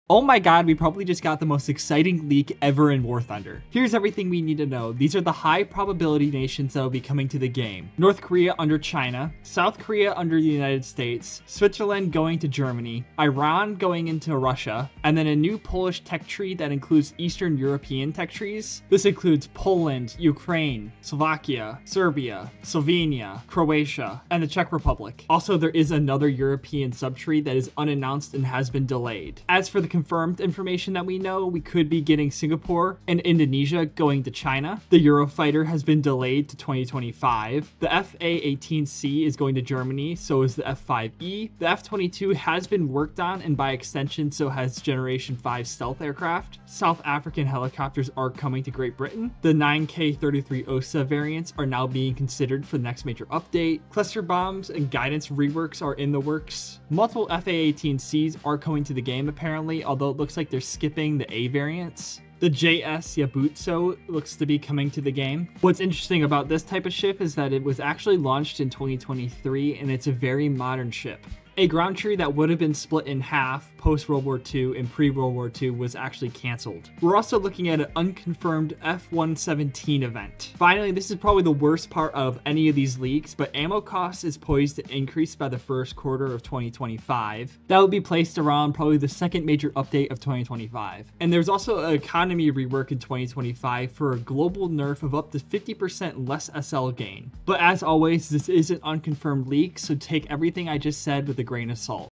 This Leak Has Everything We Did And Didn't Want
Here I go over yet another War Thunder Leak: This one is massive. We could see everything from the F22, to the Eurofighter soon, along with 13 new nations (maybe). Yes Poland is included.

Discord.gg/nbla